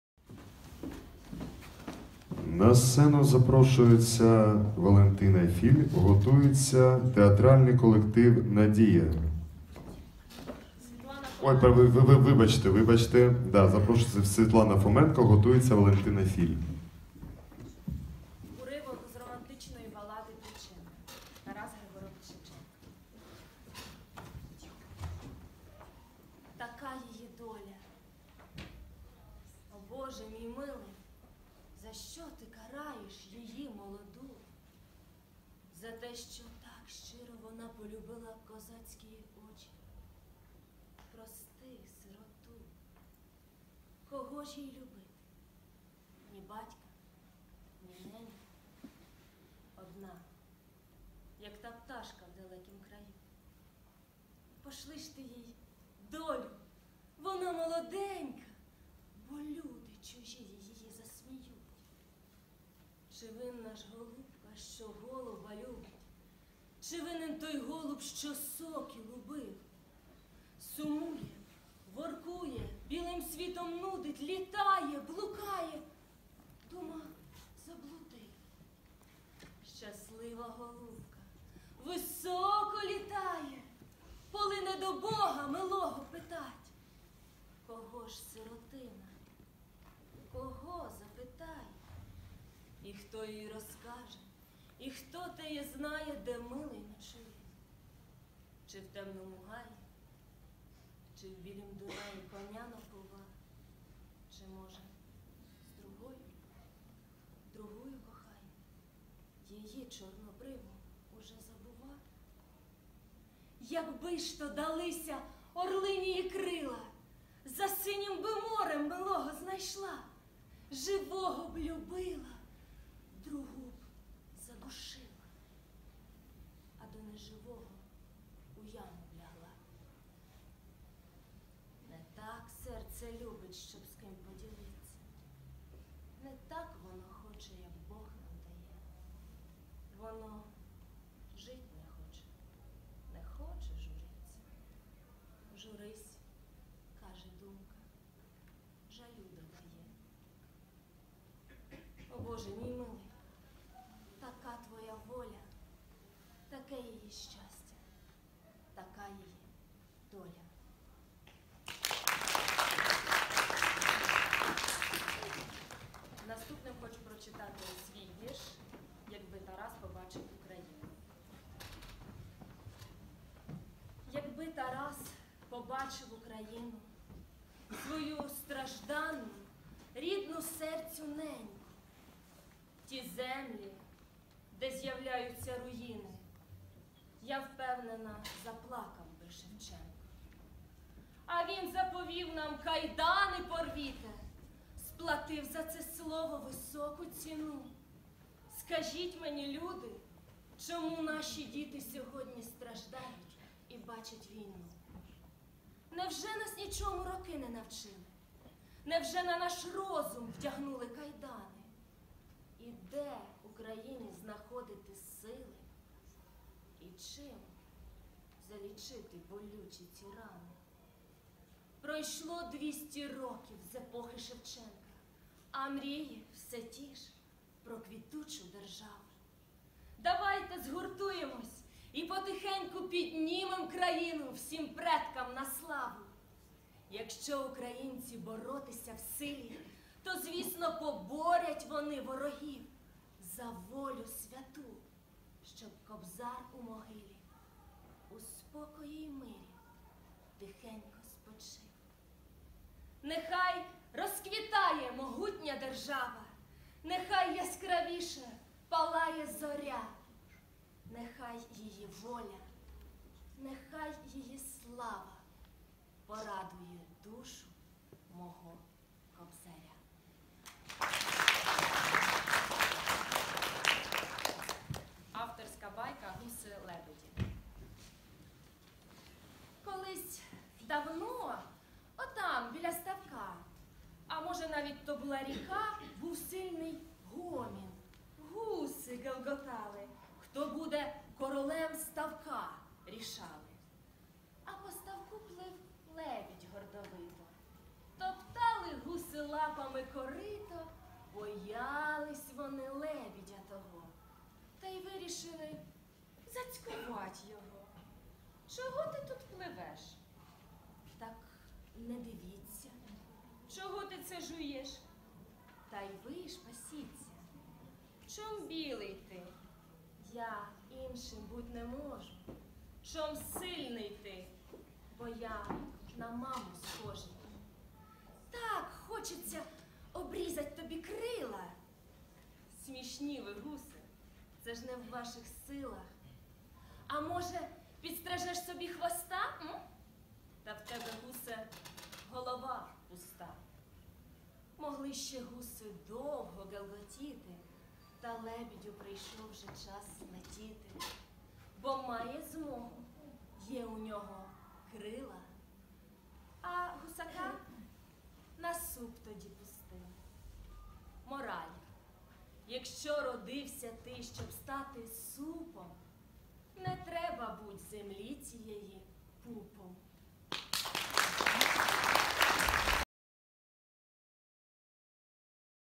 Світлана Фоменко на обласному конкурсі читців "В сім'ї вольній, новій"
ІІ місце! Ура!!!